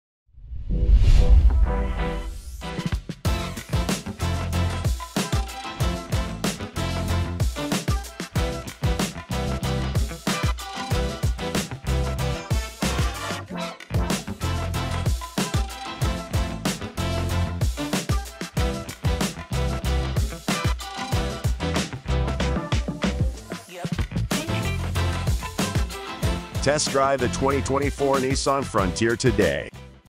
2024 Nissan Frontier Review | St. Charles Nissan
Unleash the power of adventure with the 2024 Nissan Frontier! Join us for an in-depth review as we explore its rugged design and off-road prowess.